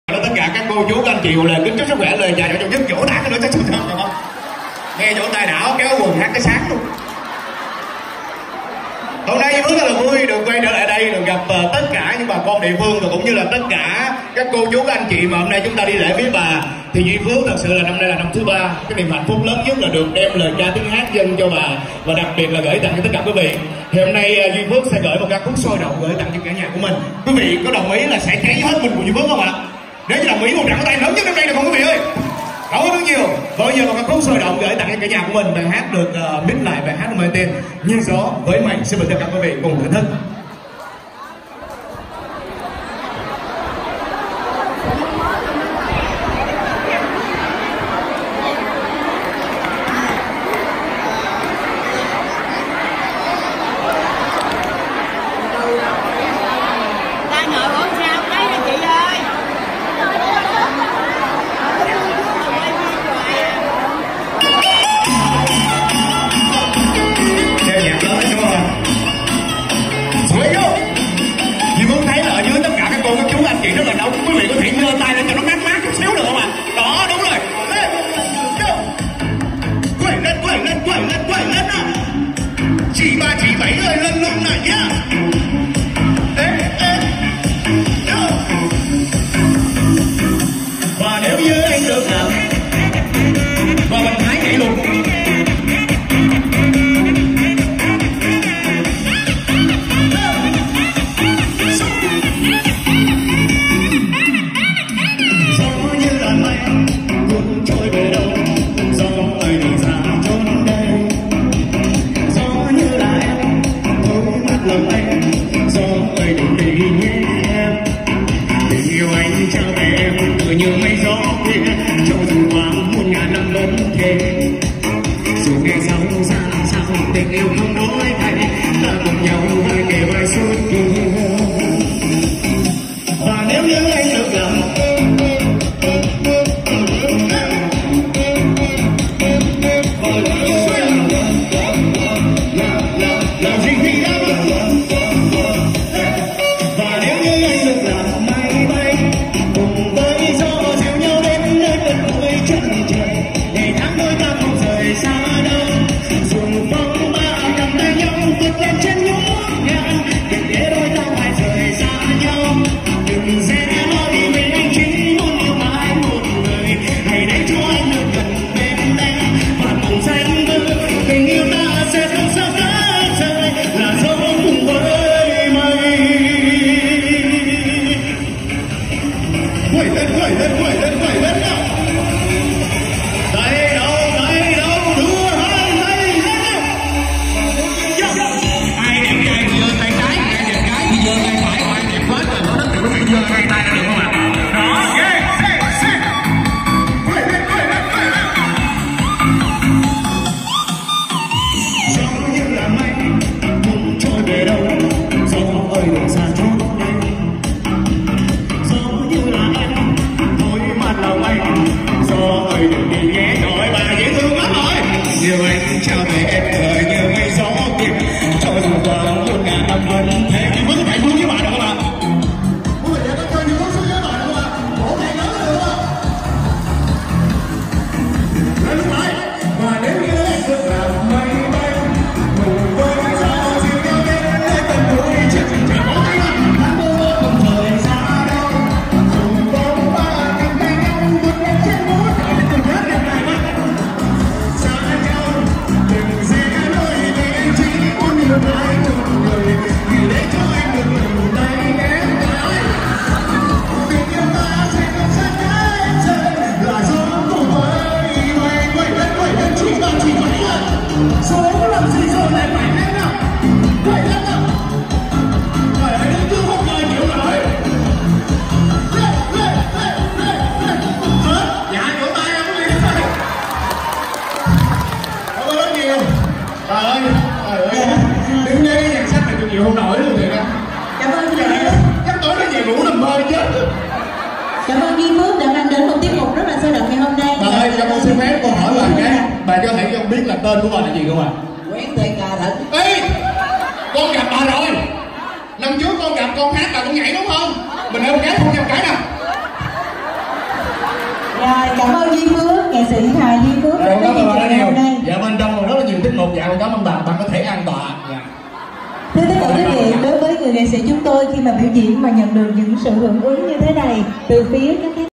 Duy Phước gặp 3 Cụ bà U50,U60,U70 chịu chơi khiến ai cũng sửng sốt tại chùa bà CHÂU ĐỐC
Chùa Bà Châu Đốc, An Giang
Cầu chúc cho tất cả mọi người luôn được bà phù hộ, được nhiều sức khoẻ, làm ăn phát đạt và thành công trong cuộc sống
Duy Phước mến Chúc mọi người luôn luôn may mắn và hạnh phúc nhé.
Lễ bà ngày 23,24/4 âm lịch hằng năm
Chương trình văn nghệ 27,28/4 âm lịch hằng năm
Cả nhà có dịp đến vía bà và xem chương trình văn nghệ nhé